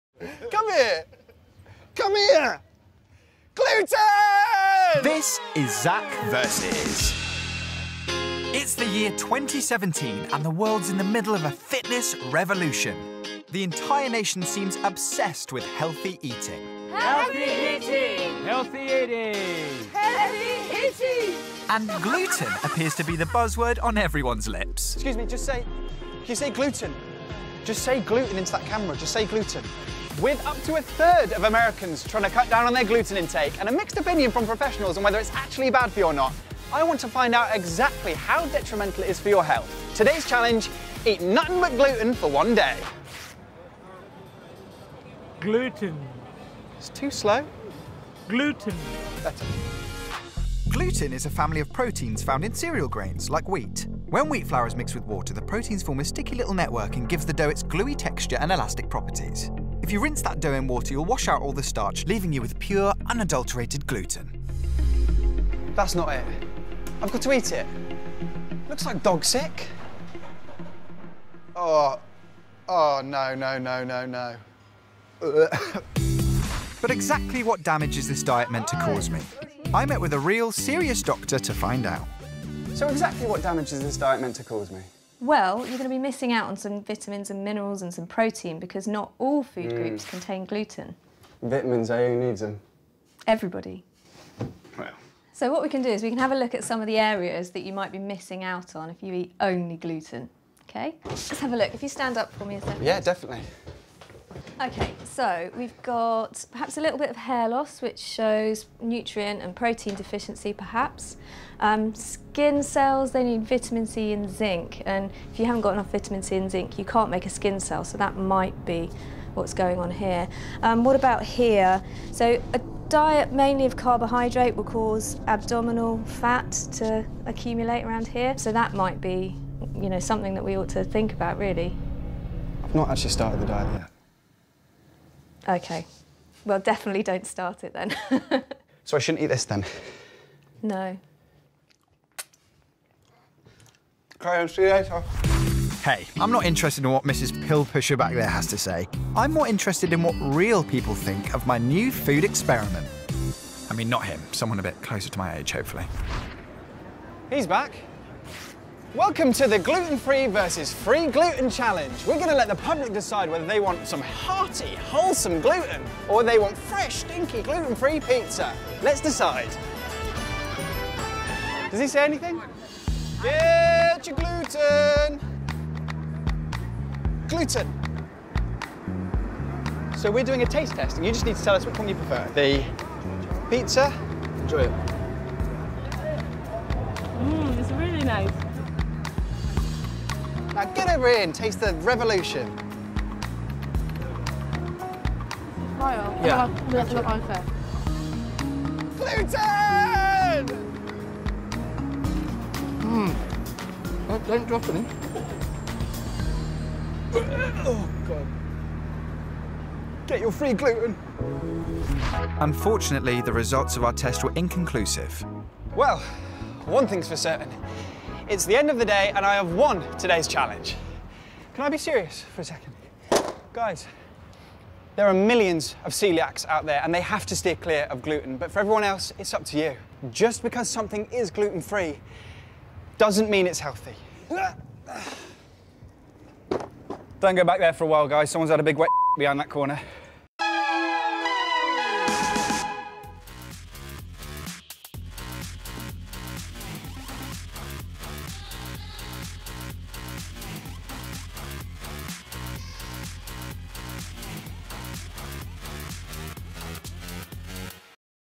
Zac Vs Gluten
Can you survive an entire day eating nothing but gluten? Zac Fox investigates…